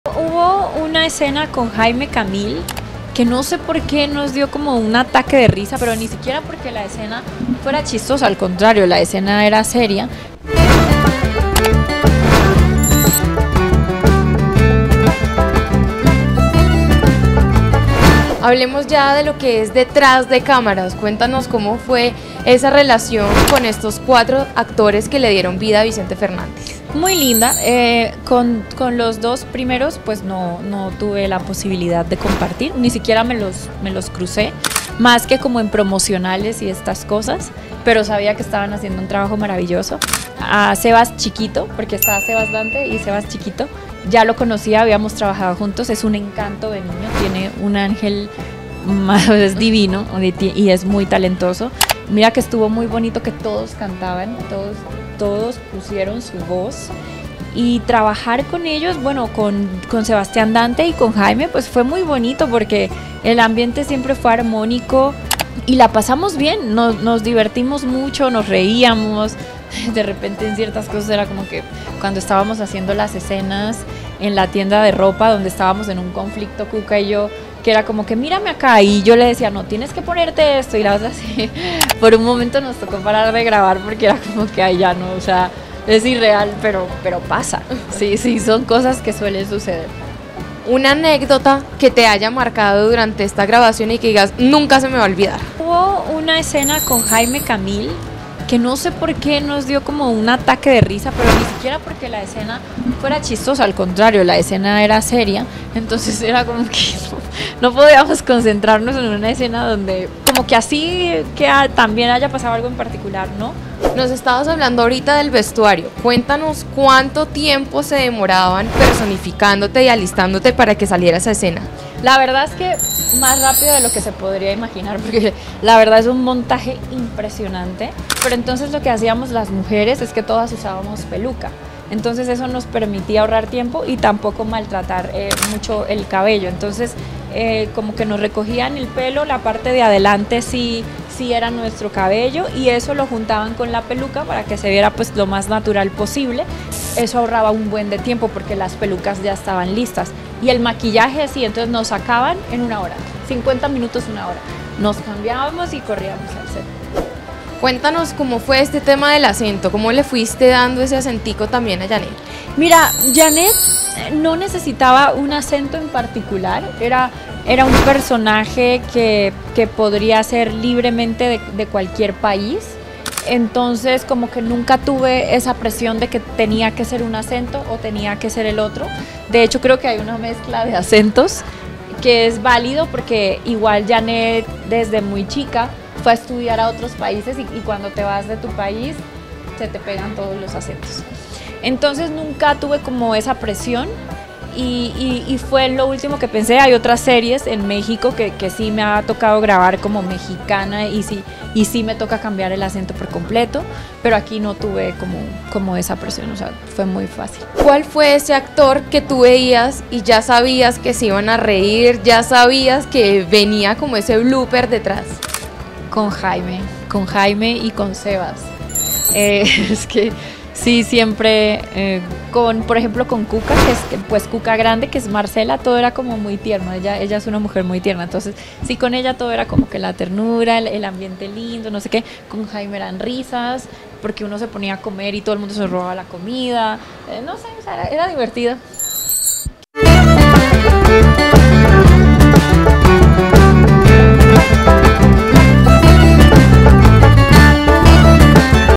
Camila Rojas revela algunos detalles detrás de cámaras de El Rey, Vicente Fernández
La actriz colombiana confiesa cómo era la convivencia especialmente con dos actores, con quienes pudo compartir la mayoría del tiempo durante las grabaciones y que eran los encargados de sacar las risas del elenco.